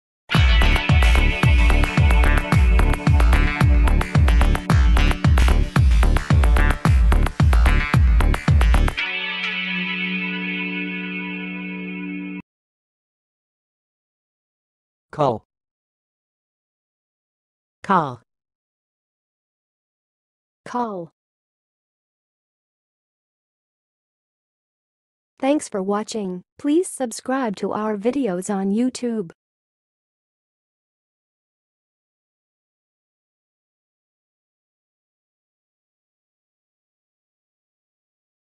How to say "kaale"! (High Quality Voices)
Watch in this video how to say and pronounce "kaale"!